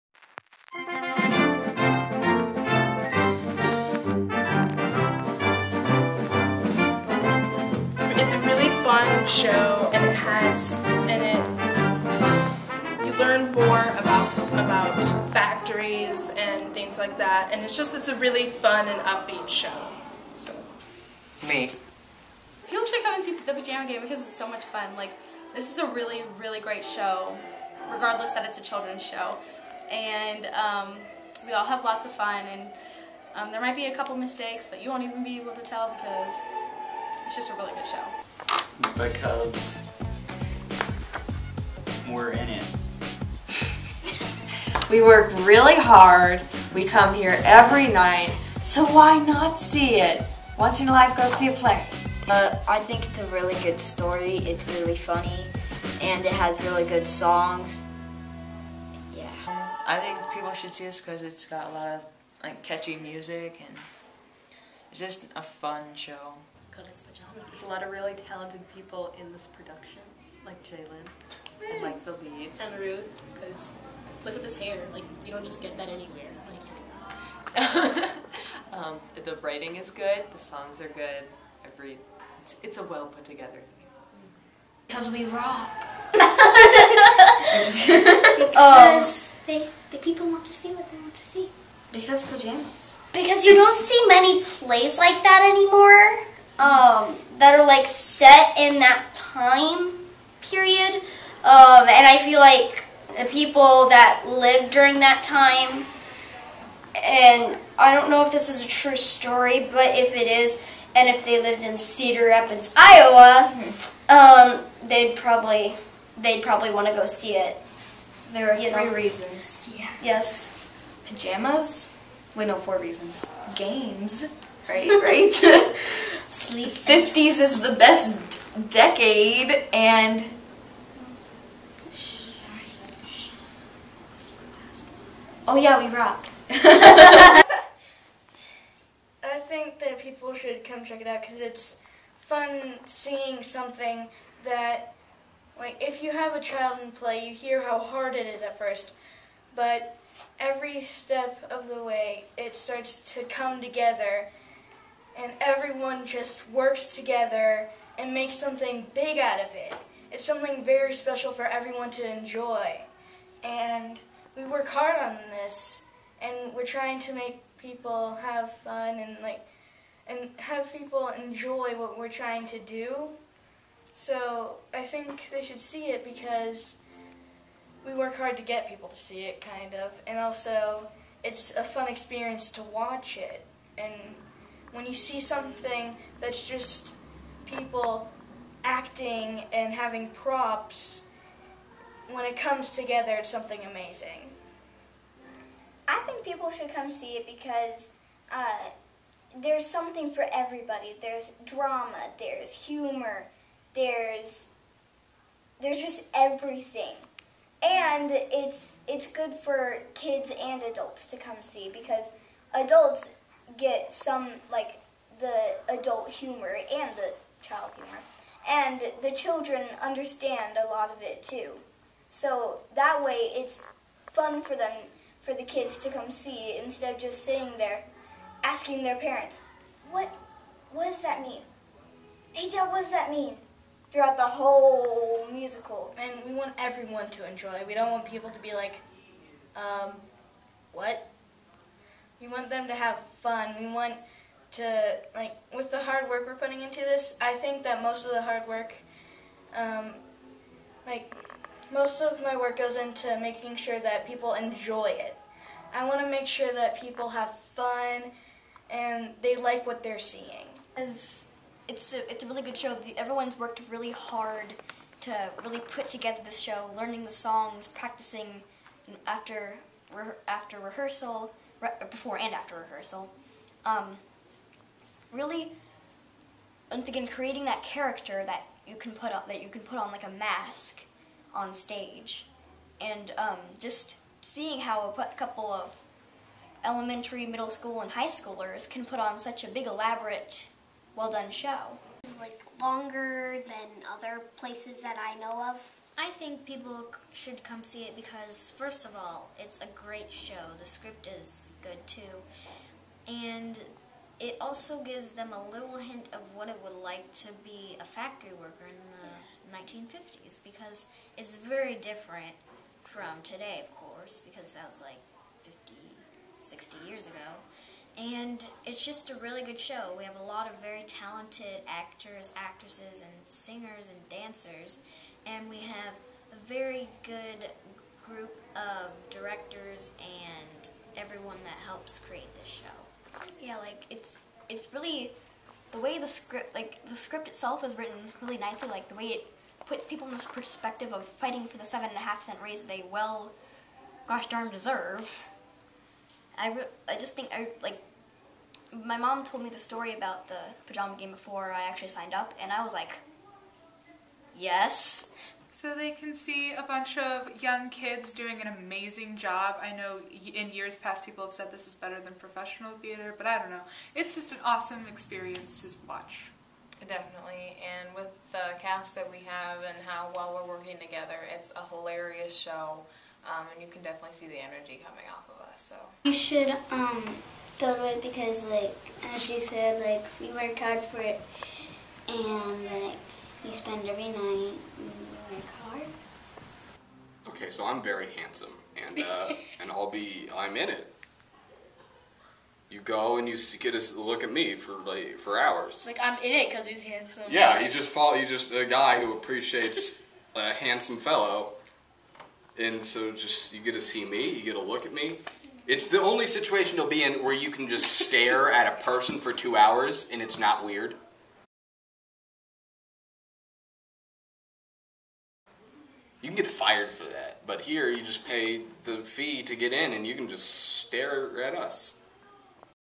Why Should You See The Pajama Game?
The cast of "The Pajama Game" give their explanations for why you should come see the show.